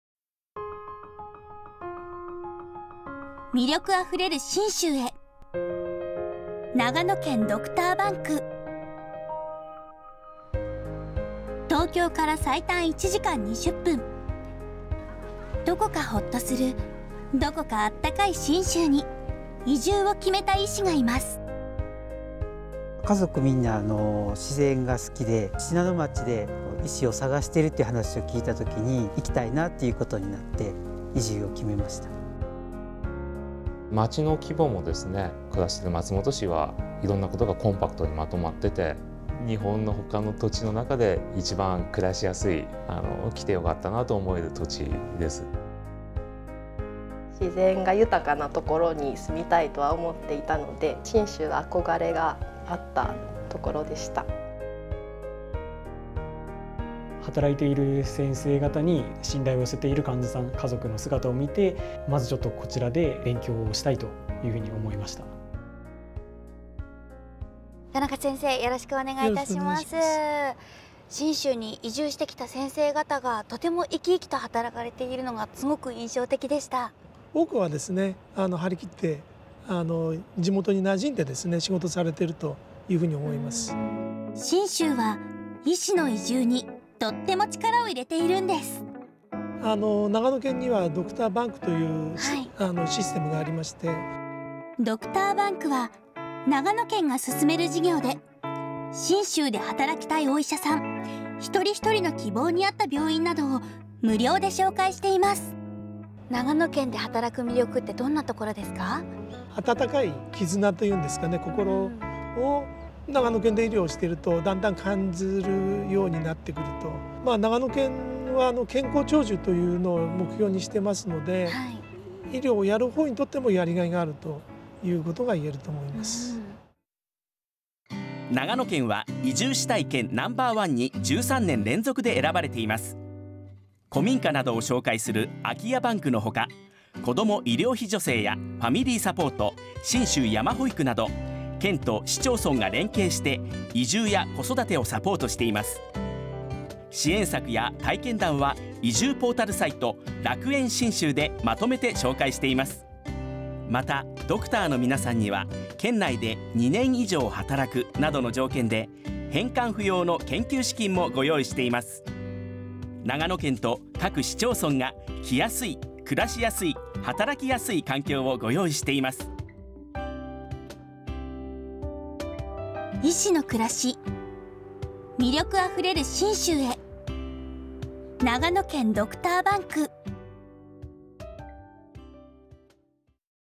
長野県ドクターバンク 長野県で働く医師のみなさん×新田恵海
長野県の取組「長野県ドクターバンク」では
医師の無料職業紹介を行っています。

美しく豊かな自然に抱かれた信州で、
あなたの知識・技術・経験を活かしませんか?

この「長野県ドクターバンク」紹介動画では
長野県出身の人気声優・新田恵海さんが
長野県に移住した医師の皆さんをご紹介しています。

移住関連雑誌のアンケートで13年連続「移住したい県」No.１ に選ばれる長野県は、美しい自然と、豊かな文化に恵まれ平均寿命は全国トップレベル、県民一人当たりの医療費も低く「健康長寿」を誇っています。

そのためにも「医師の確保」は重要です。

「いま暮らす地域で、安心して医療を受けたい」

そんな地元の方々の想いを受け止め
長野県は医師確保対策に積極的に取り組んでいます。

●ドクターバンク（医師無料職業紹介事業）

●総合医の養成支援

●医学生修学資金の貸与や各種助成制度 等

長野県は医師の皆さまの「自分らしい働き方」を全力で応援しています。

自然豊かな信州の地で

地域の健康を守るため

お力を貸してください。